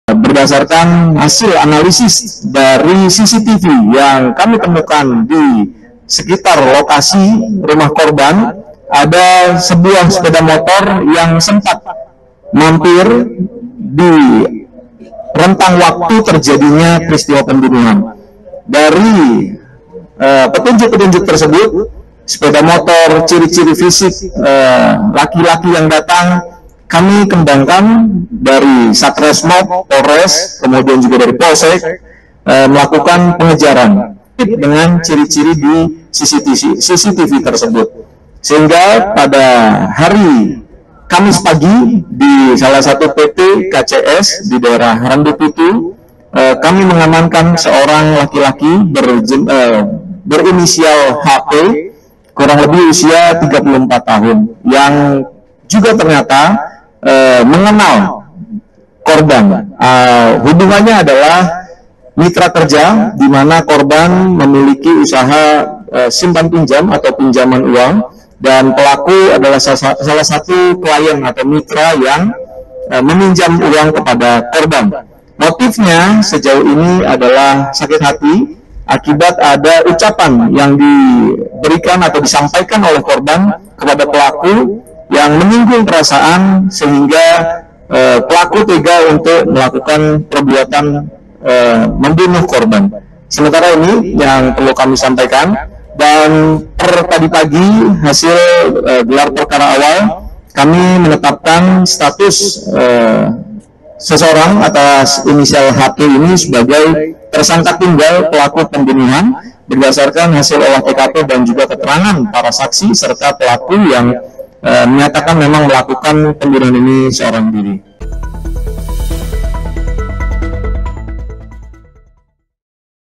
Motif Pembunuhan Ini Karena Tersinggung Perkataan Korban Saat Ditagih Hutangnya
VP : Akhmad Yusuf  / Rep : Galih Lintartika

SURYAMALANG.COM - PASURUAN

Heru Purnomo (34), warga Desa Randupitu hanya bisa pasrah saat ditangkap anggota Satreskrim Polres Pasuruan di tempat kerjanya.

Heru tidak bisa berkelit dihadapan anggota kepolisian. Dia mengakui perbuatannya membunuh mantan teman kerja dan tetangganya yakni Endang Sukowati.

Bapak satu anak ini memiliki tanggungan atau pinjaman hutang kepada korban sebesar Rp 4 juta. Sekadar informasi, korban ini memiliki usaha simpan pinjam uang.

“Korban ini sering menghubungi pelaku untuk segera membayarkan tanggungannya atau hutangnya,” kata Kapolres Pasuruan AKBP Bayu Pratama Gubunagi.

Disampaikan Kapolres, awalnya, pelaku ini merespon penagihan itu dengan biasa. Hingga pada titik, ada perkataan korban yang memancing amarah pelaku.

“Korban juga sempat menyampaikan "Wong istrimu umroh saja bisa, masak bayar hutang gak mampu, jual saja istrimu untuk bayar hutangmu,” lanjut Kapolres.

Selain itu, kata Kapolres, pelaku juga tersinggung kepada korban karena melibatkan orang lain yang diminta untuk menagihkan uang pinjaman itu ke pelaku.

“Jadi, korban menyuruh orang lain yang juga masih satu desa untuk menagih uangnya ke pelaku,” sambung Bayu, sapaan akrab Kapolres.

Perkataan dan cara penagihan korban inilah yang memancing amarah pelaku. Hingga akhirnya, pelaku tersirat untuk menghabisi korban.